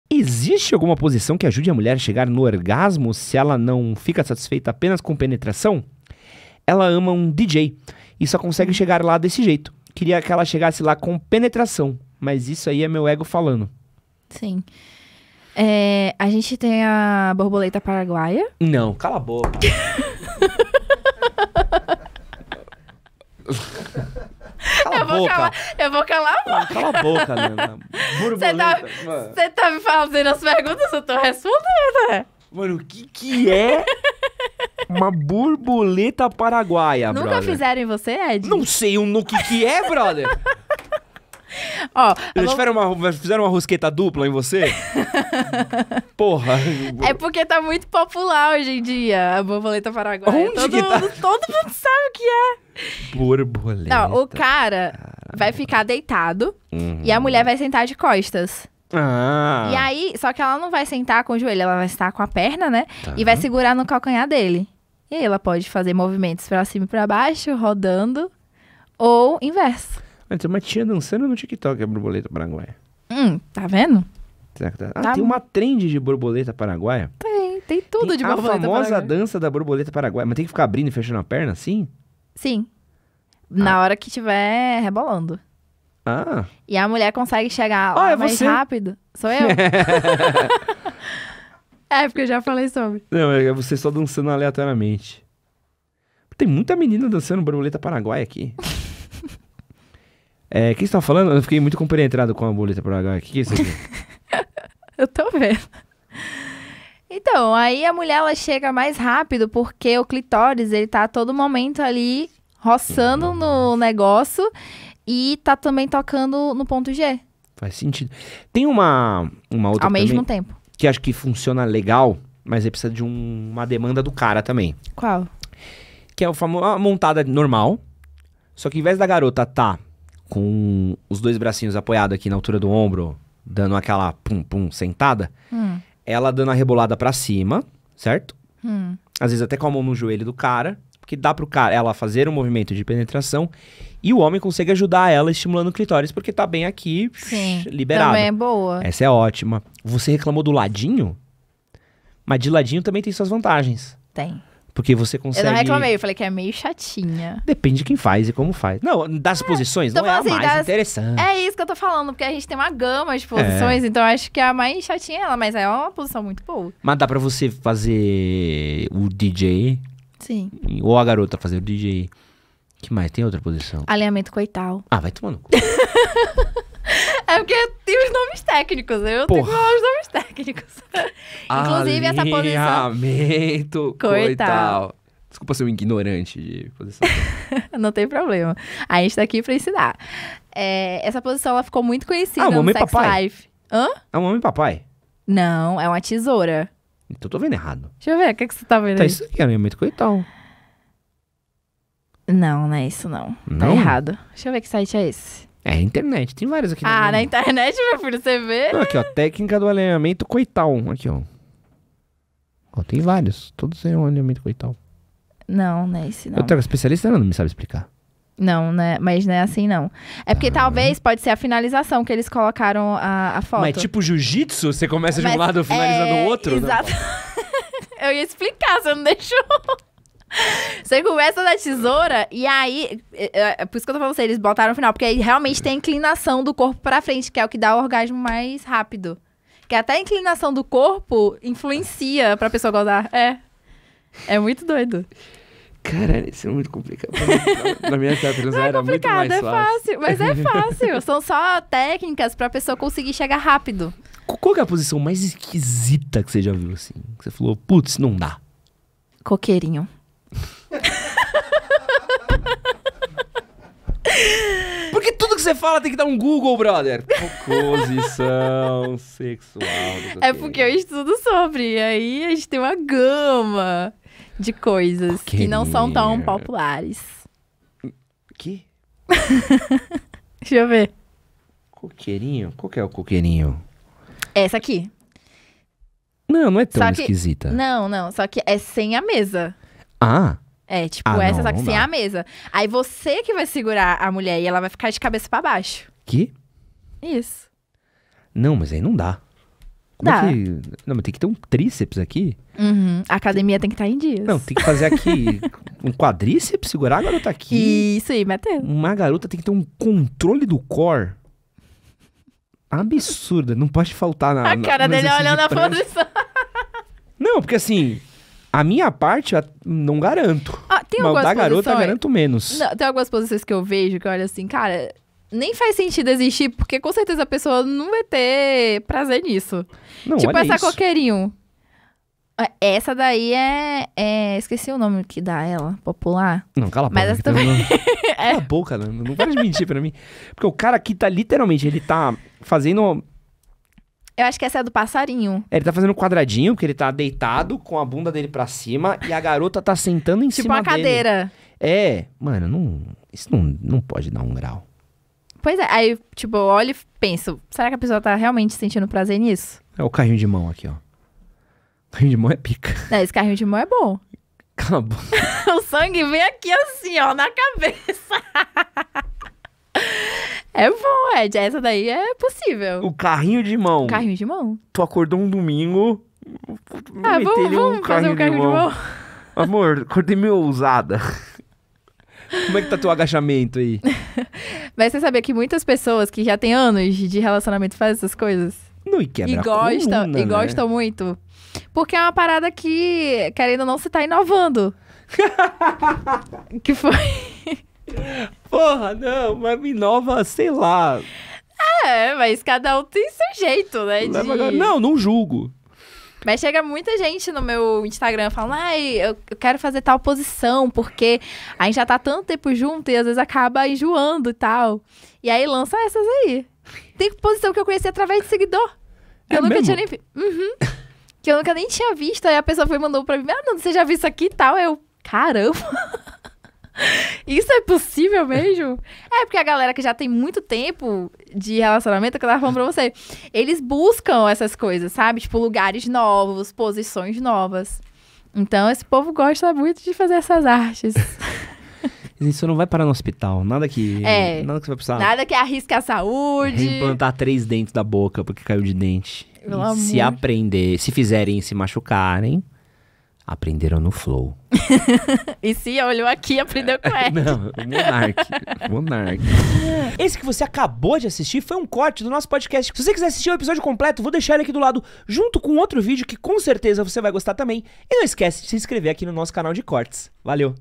AS POSIÇÕES que as MULHERES MAIS GOSTAM no SEXO! (com Garota Bolada) | PODCAST do MHM
Quais as melhores posições sexuais para a sua parceira ficar doidinha? Como fazer ela gozar na hora da penetração? Por que existem posições melhores para uma mulher gozar na penetração?

No CORTE do MHM de hoje, a influenciadora, @oficialfernandalopes  lista quais são as melhores posições sexuais para as mulheres.